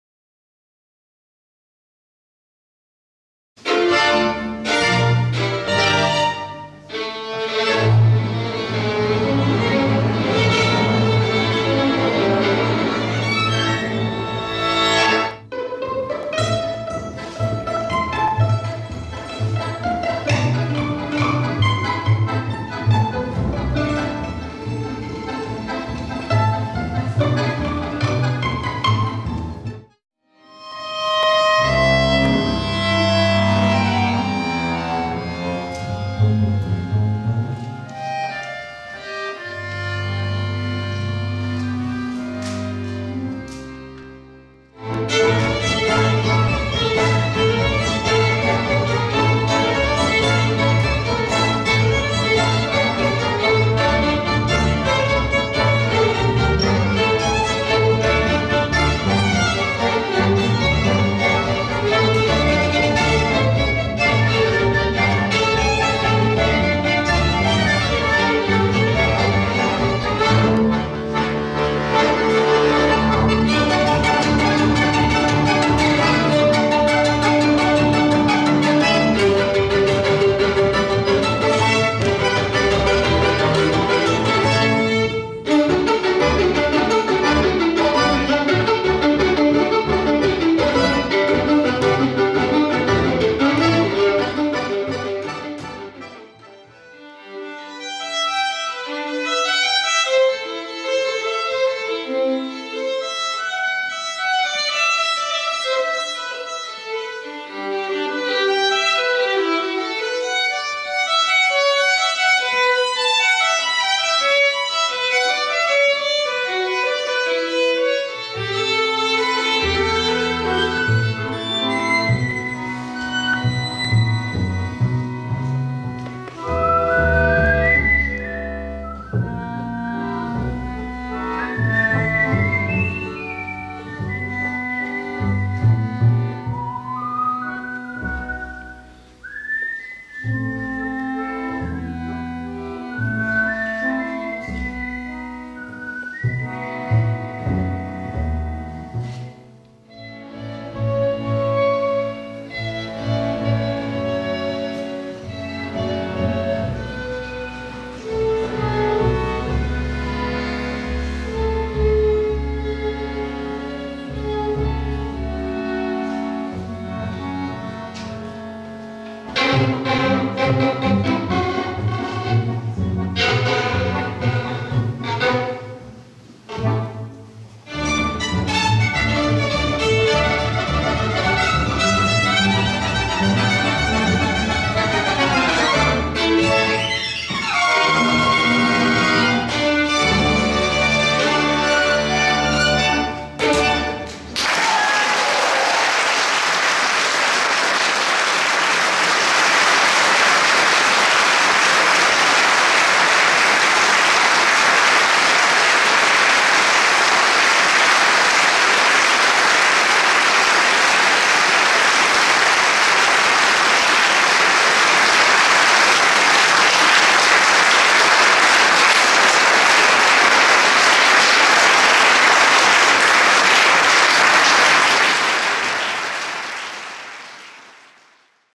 Festival des chapelles 2016 Ensemble Nomad'Lib 2016 07 16
Concerts-randonnée à Saint Julien-en-Vercors et Saint-Martin-en-Vercors avec l'Ensemble Nomad'Lib. La musique d'Europe centrale et tzigane dans une maîtrise éblouissante, une émotion et une énergie débordantes. Tout l'art de faire parler le violon et de faire pleurer les âmes.